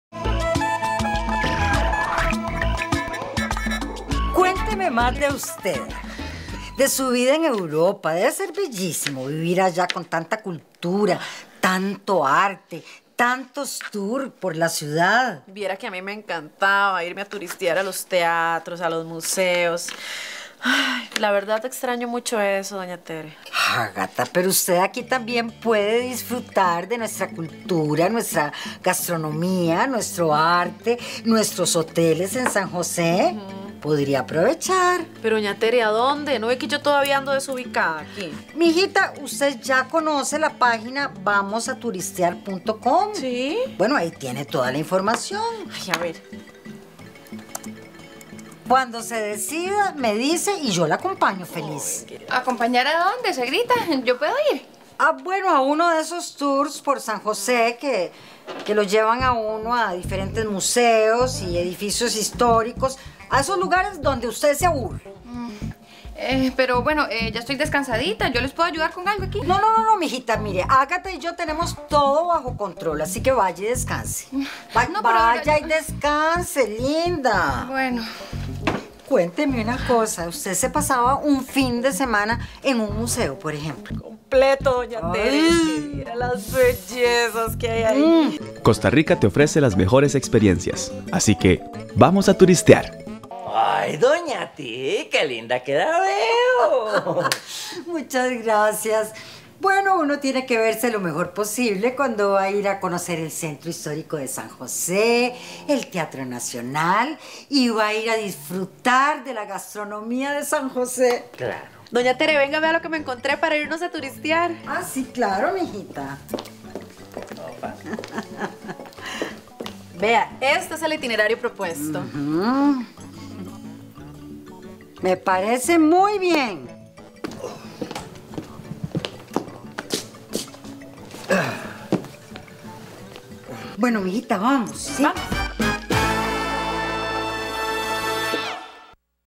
Doña Tere te invita a vivir una nueva experiencia cultural
Si no sabés que donde turistear, Doña Tere te recomienda vivir la experiencia de disfrutar de la gastronomía de San José y su cultura.